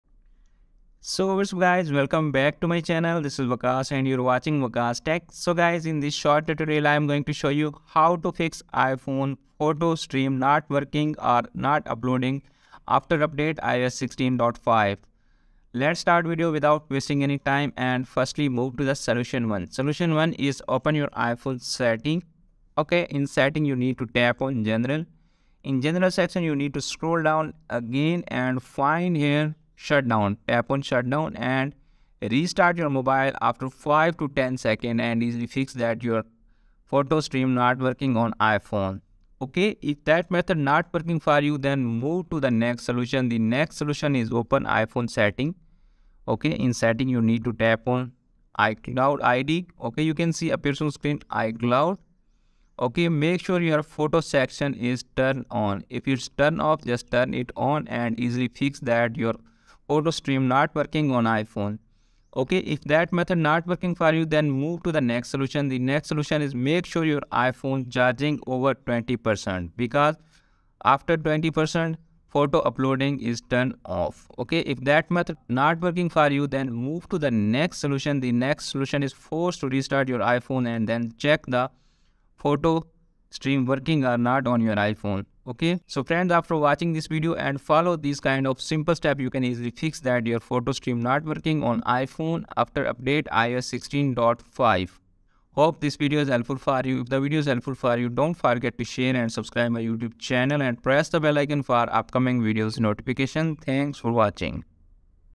Fix Photos Not Syncing Or Working After Update iOS 16.5 !! iPhone iCloud Photo Stream Not Syncing
In this video i will show you how to fix iPhone photos stream not working or syncing or not updating on iCloud. After watching this video you can easily fix this issue hope this video helps you a lot .